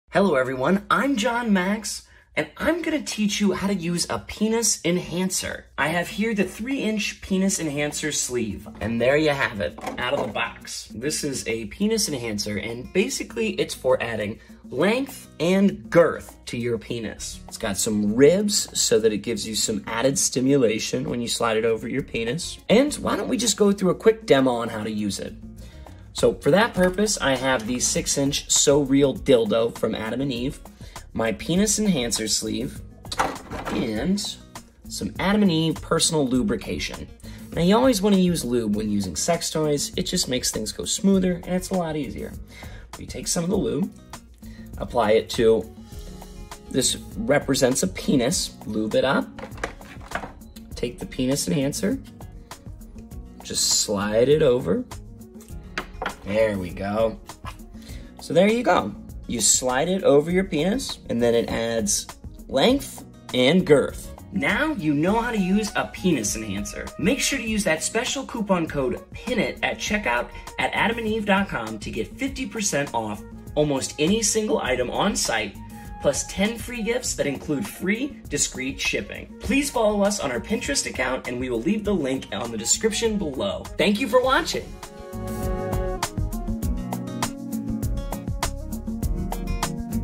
How to Use a Penis Enhancer? | Male Enhancement | Penis Extender Review
Boost your girth with this penis extender sleeve from Adam and Eve, the 3-Inch Penis Enhancer Sleeve. It has a bulbous head with veins for extra pleasure. The penis extender can indeed change your ordinary play to a much more unforgettable play with your partner; it will always provide orgasmic stimulation. Don't forget to use a lubricant.

🍎 Adam and Eve is America’s Trusted Store for Adult Products for 45 years! 🍎
✔Adam and Eve Triple Guarantee
✔No Hassle 90 Days Return
✔Discreet Billing and Shipping